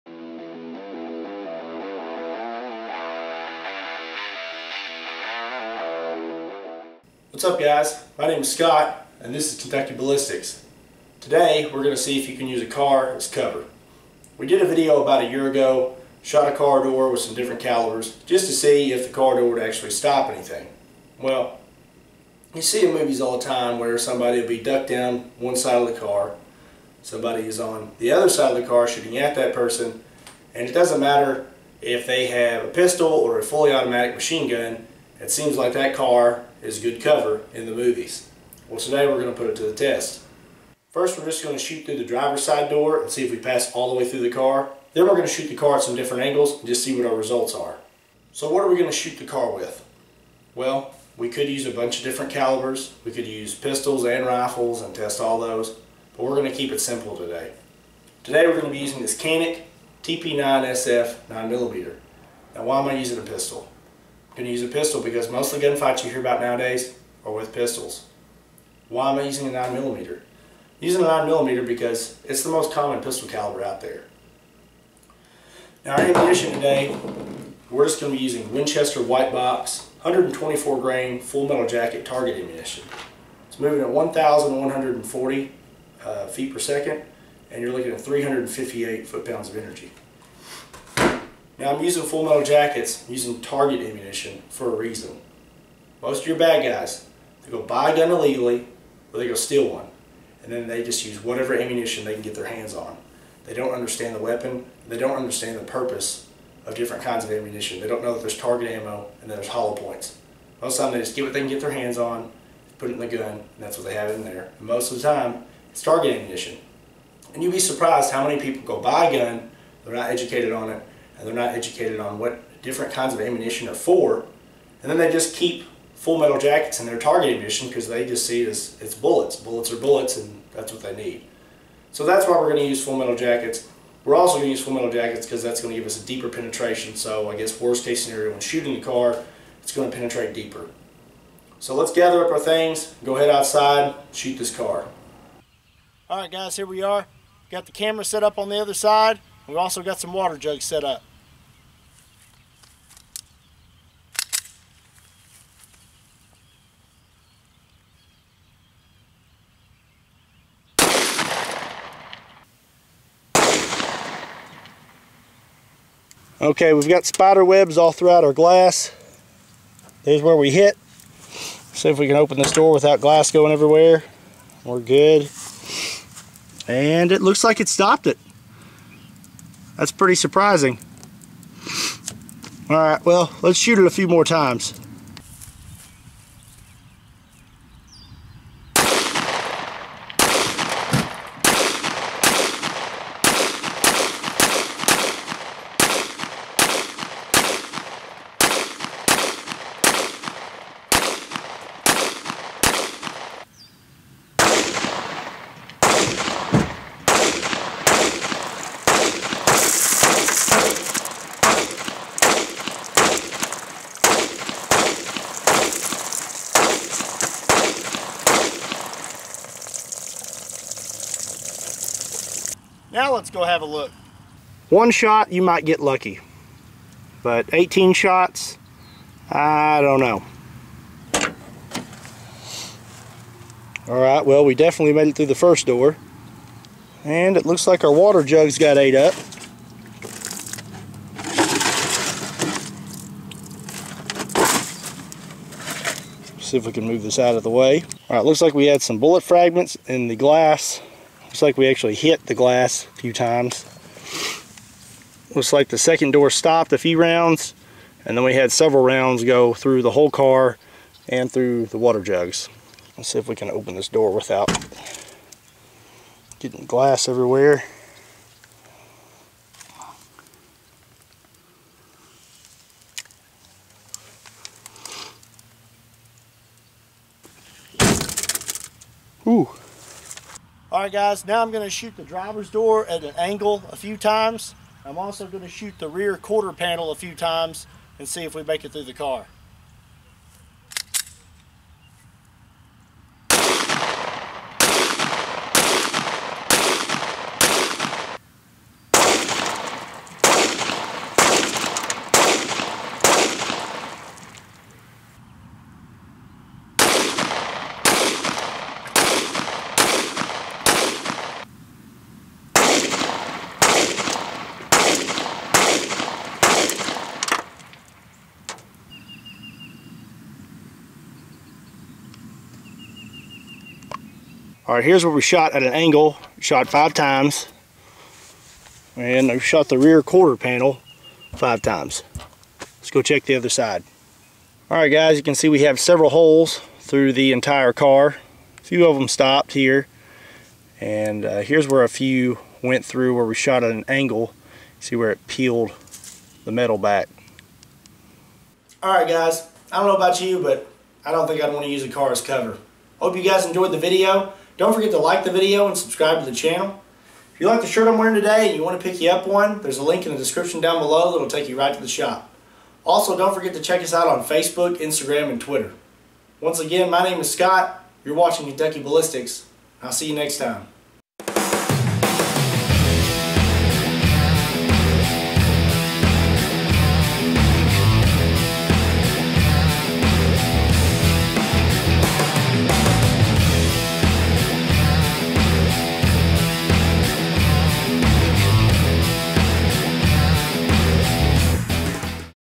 Can A Car Stop A Bullet?
Today on Kentucky Ballistics we see if a car would make good cover. It seems like cars are always bulletproof in the movies. Well today we put a car to the test. Do you think a car can stop a bullet? Watch and find out!

The gun used in this video is a Canik TP9SF 9mm. The ammo used is Winchester 9mm 124 grain full metal jacket target ammunition.

Check out Kentucky Ballistics Clothing! Also like and follow Kentucky Ballistics on Facebook, Instagram and Twitter!